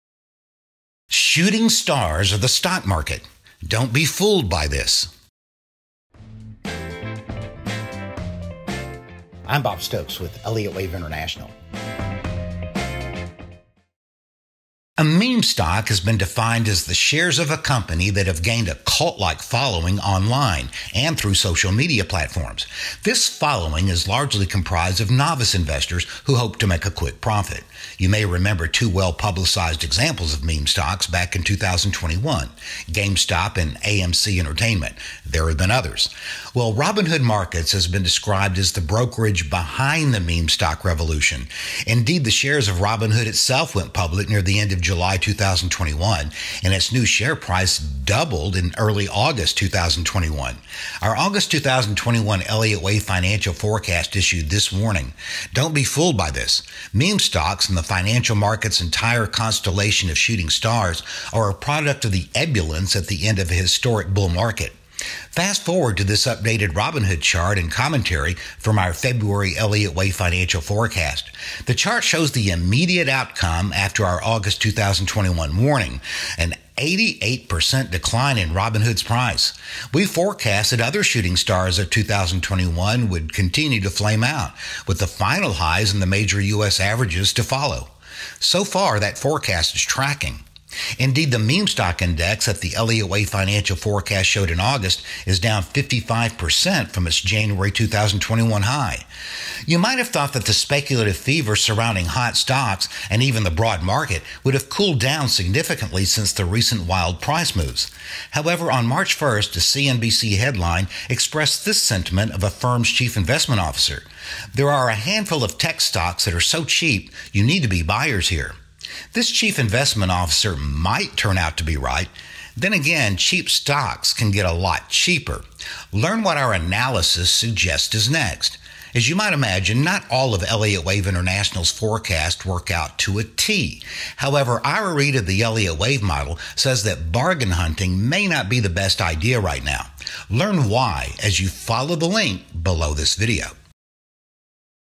“Shooting Stars” of the Stock Market: “Don’t Be Fooled by This” | Elliott Wave International
The meme stock revolution has shown that what goes up can go way down – very rapidly. Here’s a prime example.